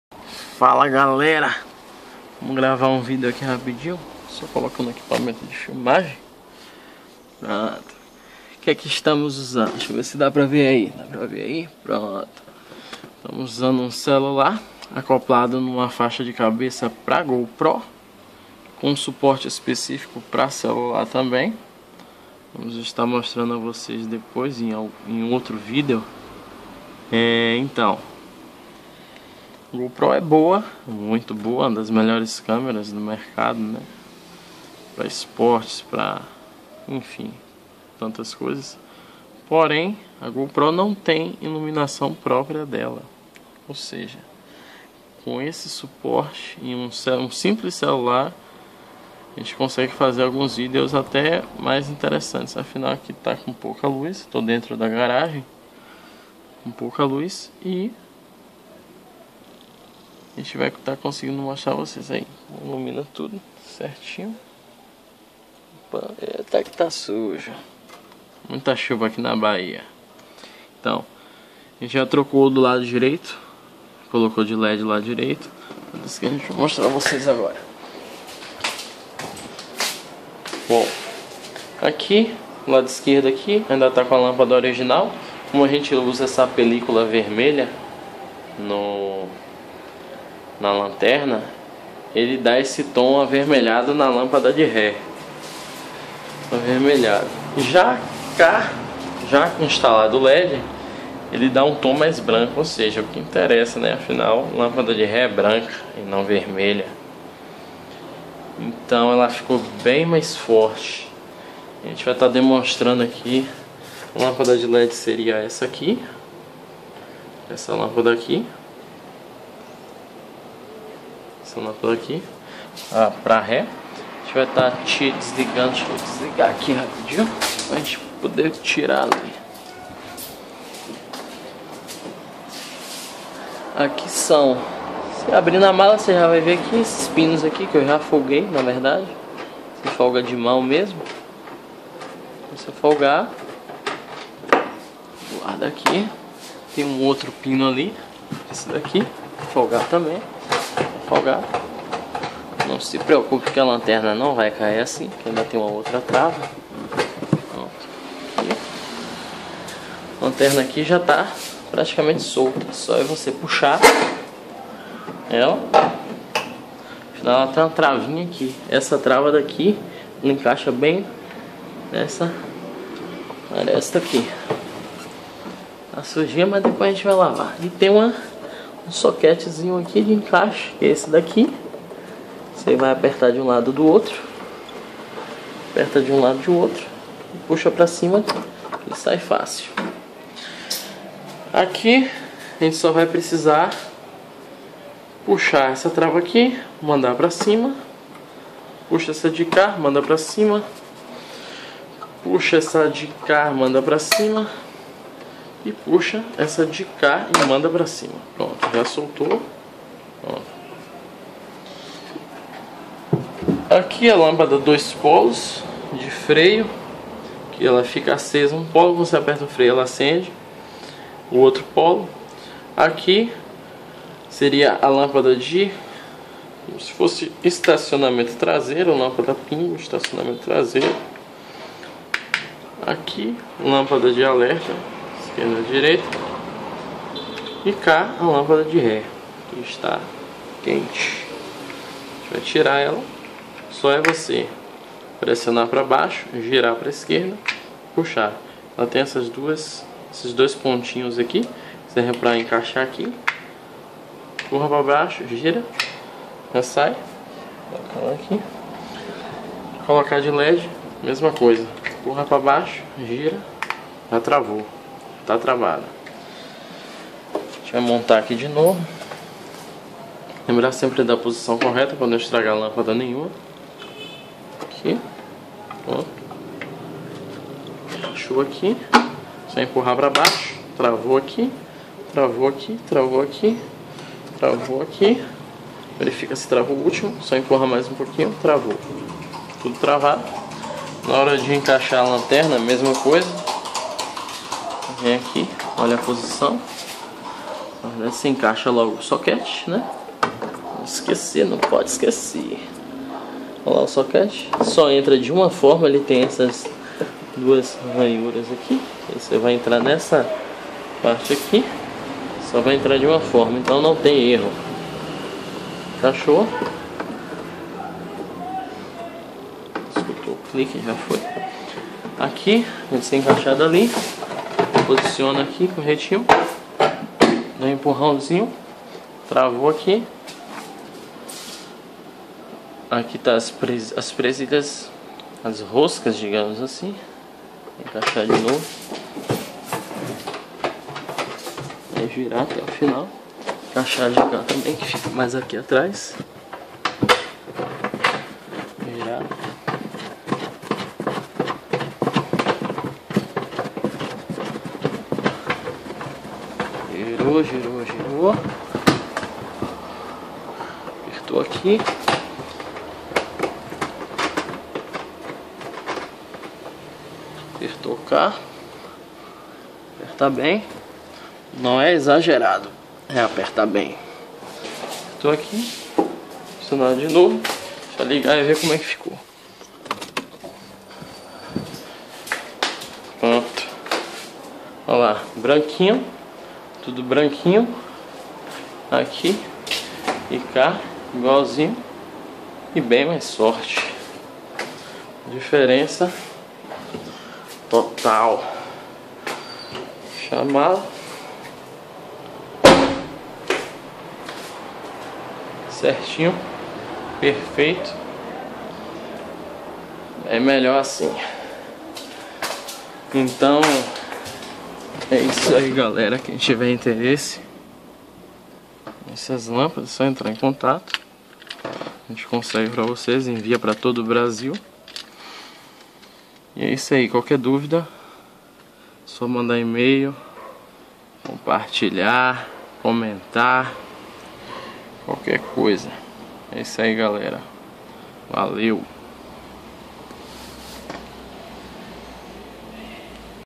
Info Tuning IT1990 | Tutorial | Instalando lâmpadas de ré LED no Gol G5
Tutorial bem rápido mostrando a instalação de lampadas led na ré do gol g5, desculpem pelo posicionamento da camera, ainda estou aprendendo a usar o equipamento de filmagem, kkkk mas mesmo assim ficou bom e dá pra entender muito bem! Valeu galera...

Gostou do vídeo? Compartilhe e clica lá no "gostei"
Se não gostou clique no compartilhar e no "gostei" também!

Segue lá no Instagram: netobandeira1